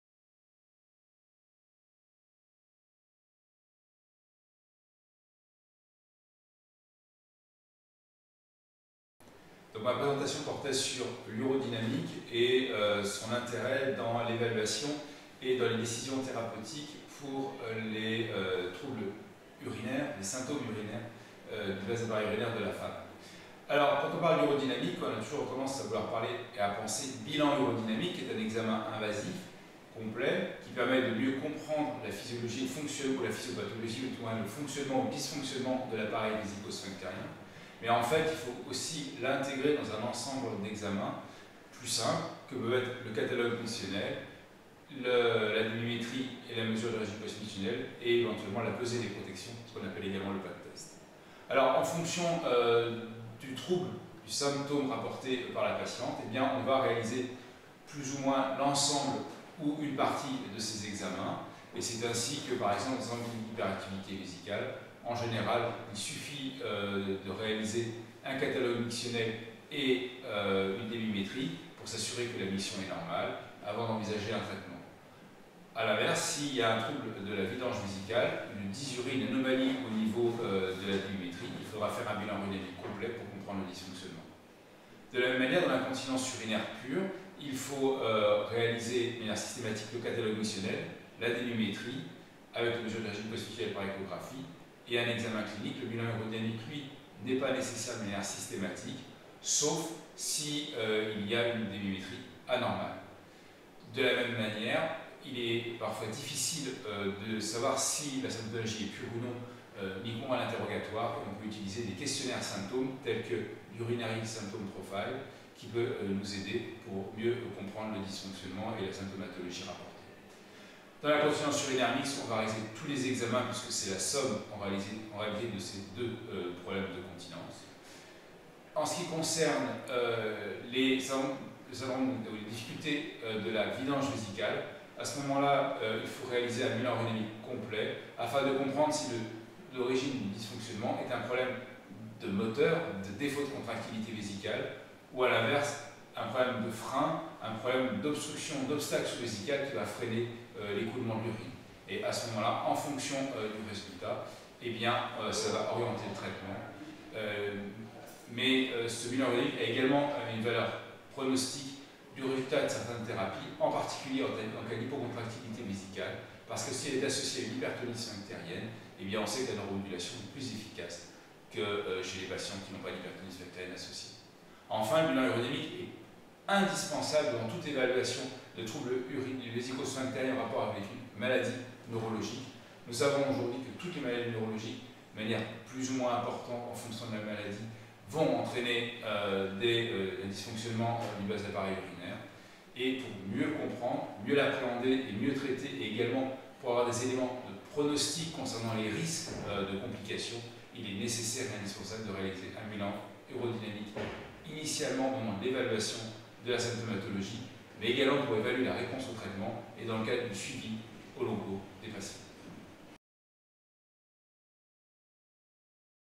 Apport de l’urodynamique des troubles de la continence et de la miction chez la femme
Apport de l’urodynamique et modalités thérapeutiques actuelles des troubles complexes de la continence et de la miction chez la femme
par Xavier GAME (Département urologie, Transplantation rénale et Andrologie, CHU Rangueil, Toulouse

Les symptômes du bas appareil urinaire sont très fréquents chez la femme. Ils nécessitent une évaluation urodynamique venant en complément de l’interrogatoire et de l’examen clinique et incluant le catalogue mictionnel, le test de pesée des protections, la débitmétrie avec mesure du résidu post mictionnel par échographie, la cystomanométrie étudiant la phase de remplissage et de vidange vésicale et l’urétromanométrie. Toutefois, tous ces examens ne sont pas systématiques et vont dépendre de la situation clinique. Ils permettent de mieux comprendre les mécanismes physiopathologiques sous-jacents et ainsi d’adapter au mieux les traitements. Dans certains cas, en particulier dans le cadre des pathologies neurologiques, ils doivent être répétés à intervalle régulier durant toute la vie de la patiente.